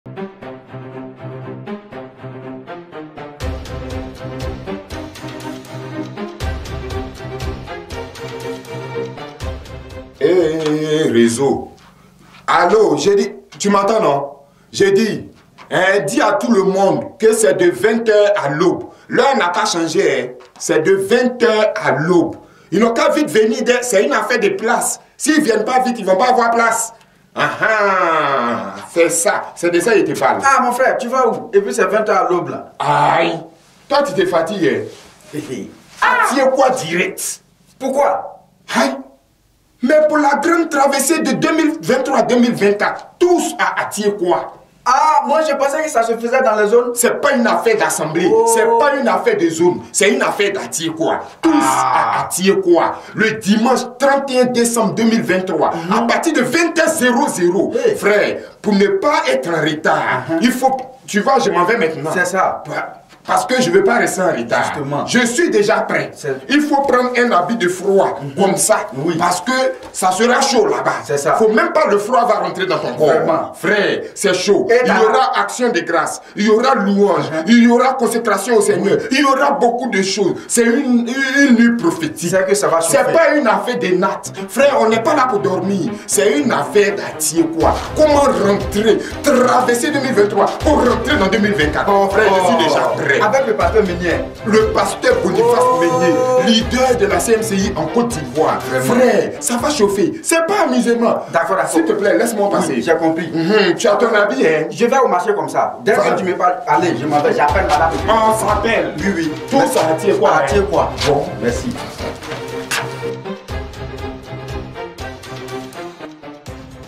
DERNIER APPEL A VENIR AU FESTIVAL DE LOUANGE
LA TROUPE THEATRALE de la CMCI CÔTE D'IVOIRE T'INVITE AU FESTIVAL DE LOUANGE ET D'ACTION DE GRACE DE CE 31 DECEMBRE 2023